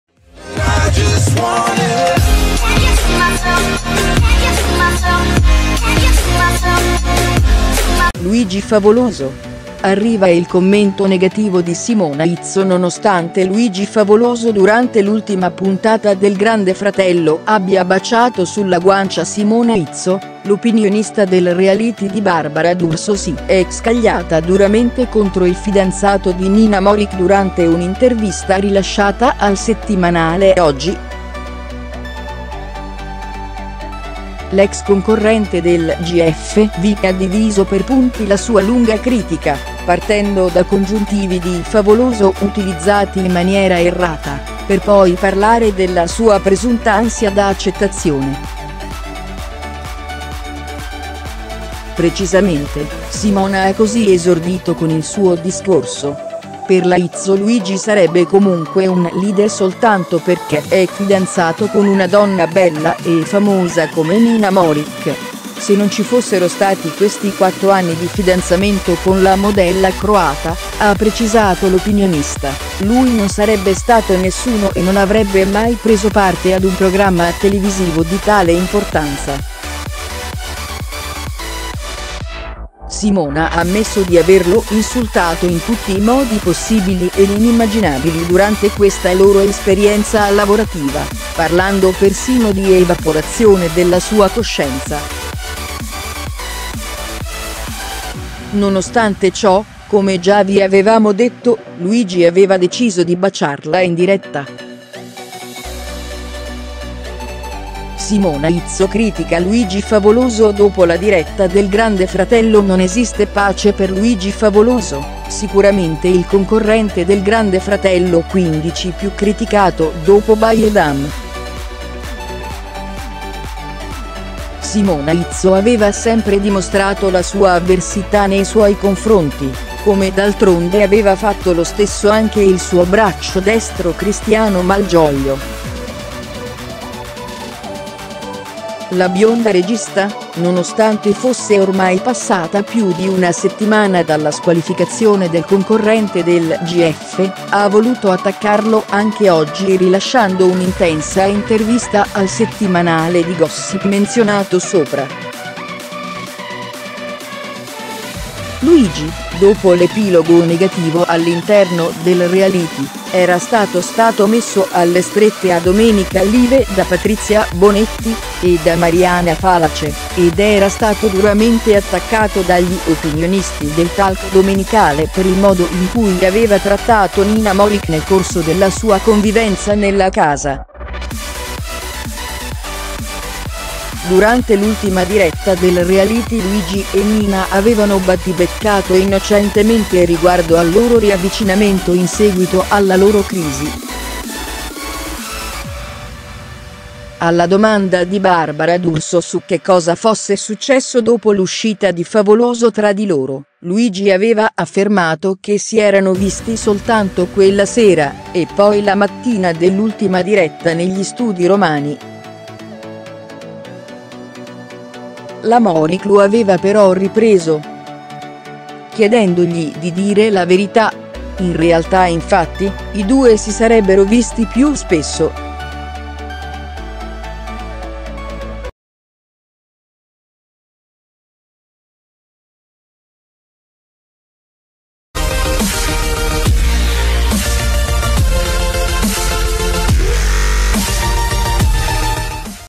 Simona Izzo si scaglia contro Luigi Favoloso: “C’è da inorridire”
Luigi Favoloso: arriva il commento negativo di Simona Izzo Nonostante Luigi Favoloso durante l’ultima puntata del Grande Fratello abbia baciato sulla guancia Simona Izzo, l’opinionista del reality di Barbara d’Urso si è scagliata duramente contro il fidanzato di Nina Moric durante un’intervista rilasciata al settimanale Oggi. L’ex concorrente del GF Vip ha diviso per punti la sua lunga critica, partendo da congiuntivi di Favoloso utilizzati in maniera errata, per poi parlare della sua presunta ansia da accettazione. Precisamente, Simona ha così esordito con il suo discorso: Per la Izzo Luigi sarebbe comunque un leader soltanto perchè è fidanzato con una donna bella e famosa come Nina Moric: se non ci fossero stati questi quattro anni di fidanzamento con la modella croata, ha precisato l’opinionista, lui non sarebbe stato nessuno e non avrebbe mai preso parte ad un programma televisivo di tale importanza. Simona ha ammesso di averlo insultato in tutti i modi possibili ed inimmaginabili durante questa loro esperienza lavorativa, parlando persino di evaporazione della sua coscienza. Nonostante ciò, come già vi avevamo detto, Luigi aveva deciso di baciarla in diretta. Simona Izzo critica Luigi Favoloso dopo la diretta del Grande Fratello Non esiste pace per Luigi Favoloso, sicuramente il concorrente del Grande Fratello 15 più criticato dopo Baye Dame. Simona Izzo aveva sempre dimostrato la sua avversità nei suoi confronti, come d’altronde aveva fatto lo stesso anche il suo braccio destro Cristiano Malgioglio. La bionda regista, nonostante fosse ormai passata più di una settimana dalla squalificazione del concorrente del GF, ha voluto attaccarlo anche oggi rilasciando un’intensa intervista al settimanale di gossip menzionato sopra. Luigi, dopo l’epilogo negativo all’interno del reality era stato stato messo alle strette a Domenica Live da Patrizia Bonetti e da Mariana Falace, ed era stato duramente attaccato dagli opinionisti del talk domenicale per il modo in cui aveva trattato Nina Moric nel corso della sua convivenza nella Casa. Durante l’ultima diretta del reality Luigi e Nina avevano battibeccato innocentemente riguardo al loro riavvicinamento in seguito alla loro crisi. Alla domanda di Barbara d’Urso su che cosa fosse successo dopo l’uscita di Favoloso tra di loro, Luigi aveva affermato che si erano visti soltanto quella sera e poi la mattina dell’ultima diretta negli studi romani. La Moric lo aveva però ripreso, chiedendogli di dire la verità: in realtà infatti, i due si sarebbero visti più spesso. SimonaIzzo, scaglia, contro, LuigiFavoloso“C’è, inorridire”